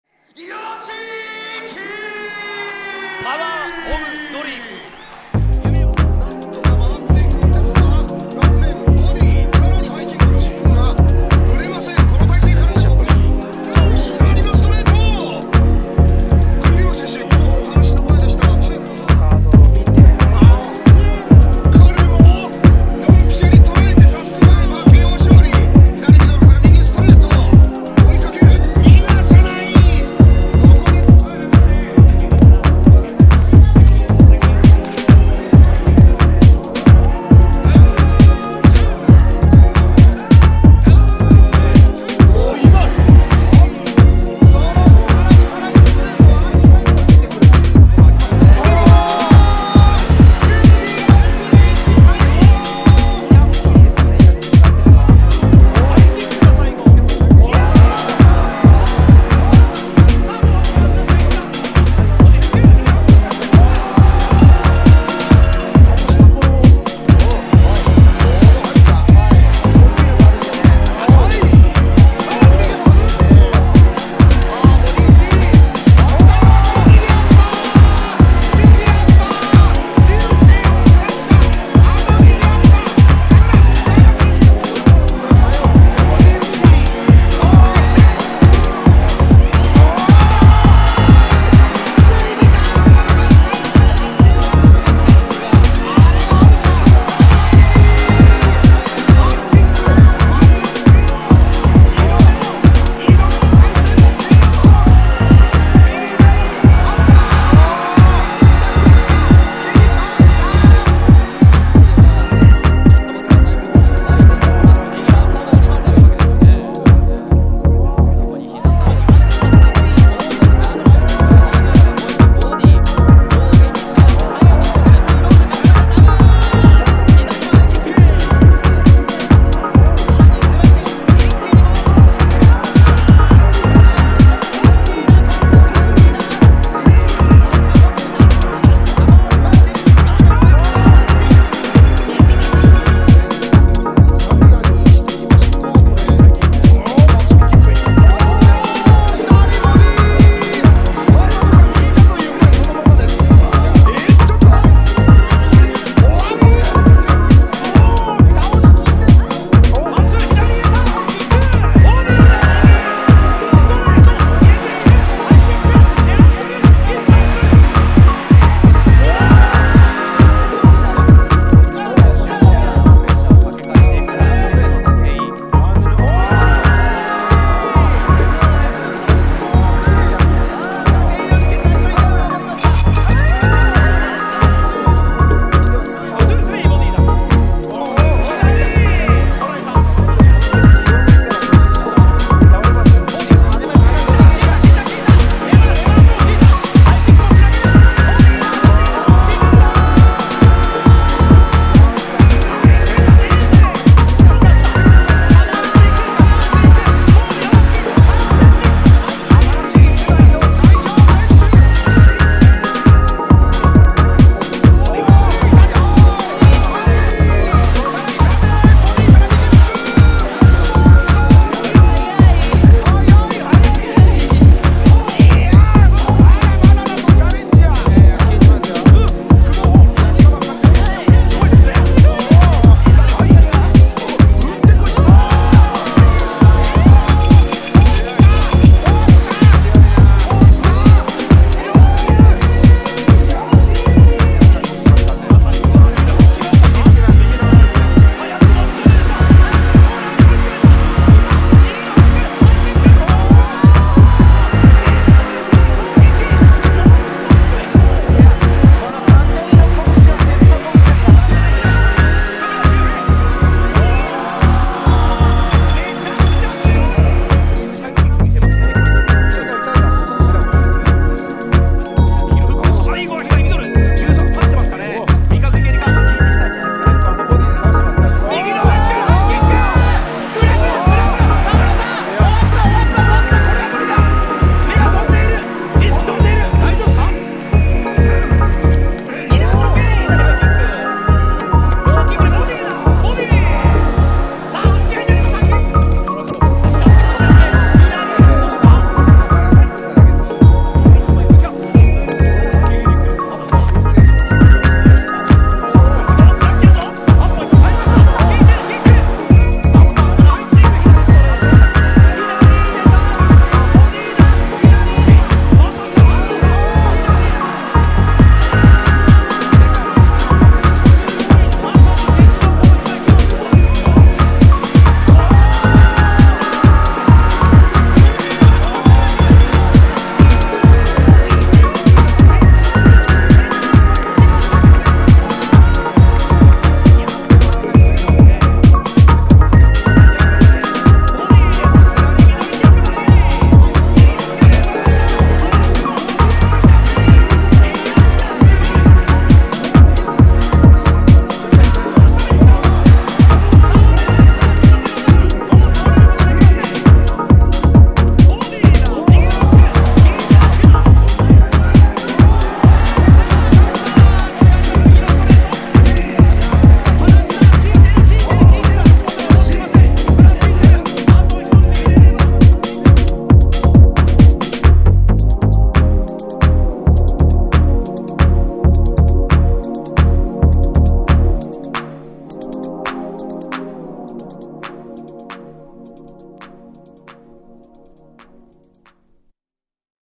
Yoshiki Takei (武居由樹) K1 Kickboxing Highlights
Yoshiki Takei (武居由樹) is a former Japanese kickboxer, fighting out of Tokyo, Japan.

As a kickboxer, he held the K-1 Super Bantamweight Championship between 2017 and 2020, as well as winning the 2017 and 2019 K-1 Super Bantamweight Grand Prix. Takei relinquished his K-1 title in December 2020 & announced his transition to boxing. At the time of his retirement, he was the #1 flyweight and #8 pound for pound kickboxer in the world according to Combat Press.